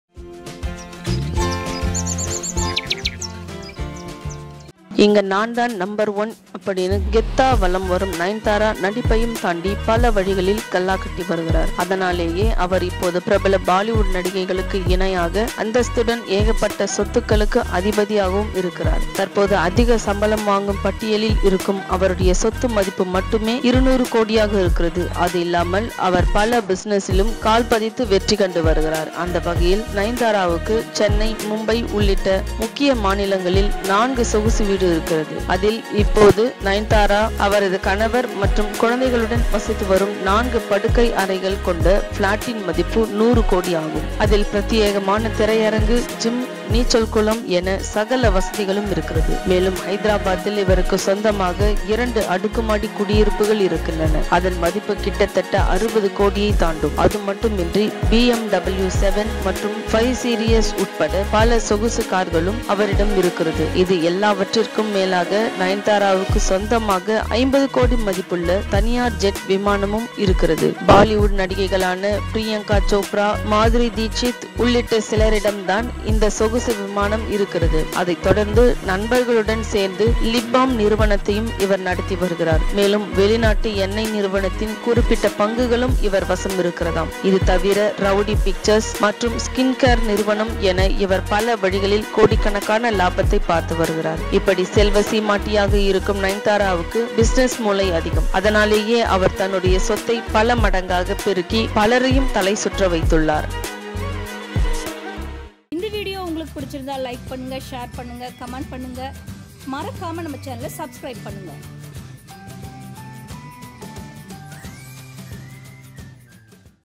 100 கோடிக்கு வீடு, ஜெட் விமானம்.. கிறுகிறுக்க வைக்கும் செல்வ சீமாட்டி நயன்தாராவின் சொத்து மதிப்பு
Nayanthara who was seen alongside Shah Rukh Khan in ‘Jawan’ is considered as one of the richest female actors of this country. She reportedly has a net worth of around Rs 200 crore. According to reports, the actress owns a home worth Rs 100 crore which is one of her four luxurious properties stretching from Tamil Nadu to Mumbai. Currently, she shares a 4 BHK flat with her husband Vignesh Sivan, with an estimated worth of Rs 100 crore. The house reportedly features exclusive amenities like a private cinema hall, a swimming pool and a multifunctional gym. Nayanthara also has two apartments in Banjara Hills, Hyderabad, each valued at around Rs 30 crore, state reports. The ‘Bigil’ actress is also a proud owner of several high-end vehicles like BMW 7 series worth Rs 1.76 crore, Mercedes GLS350D worth Rs 1 crore and a BMW 5 series as well. Nayanthara is also a part of the elite club of actresses who own a private jet. Apart from Bollywood divas like Shilpa Shetty, Priyanka Chopra and Madhuri Dixit, the 38-year-old actress also owns a private jet which is approximately valued at Rs 50 crore. Reports further stated that Nayanthara has stakes in several business ventures as well like a lip balm company and a UAE-based oil company. She also co-owns the production house Rowdy Pictures Banner along with her husband Vignesh